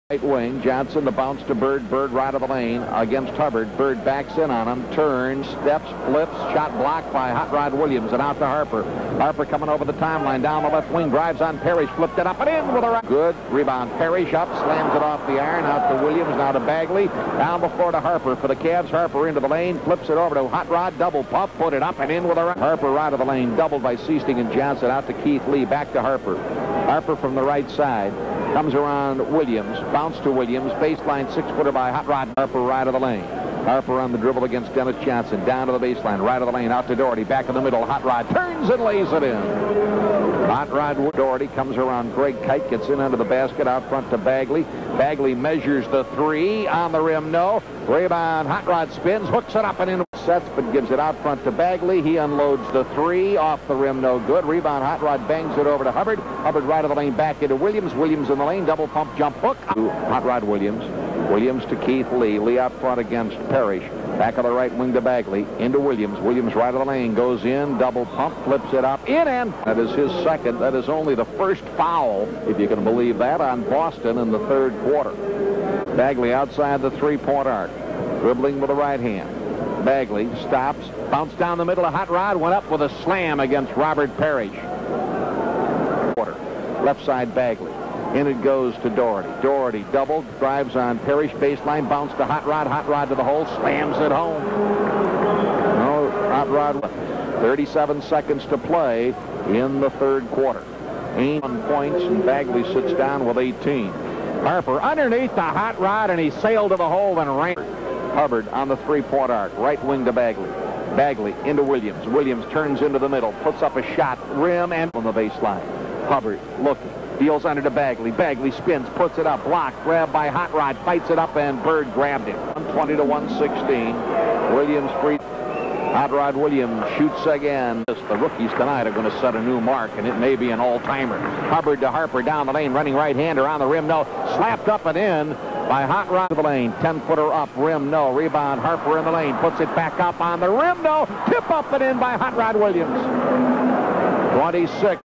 Hot Rod Williams Cavaliers 24pts 10rebs vs Celtics (1987)
John “Hot Rod” Williams enjoyed the most success of his NBA career on the Cleveland Cavaliers.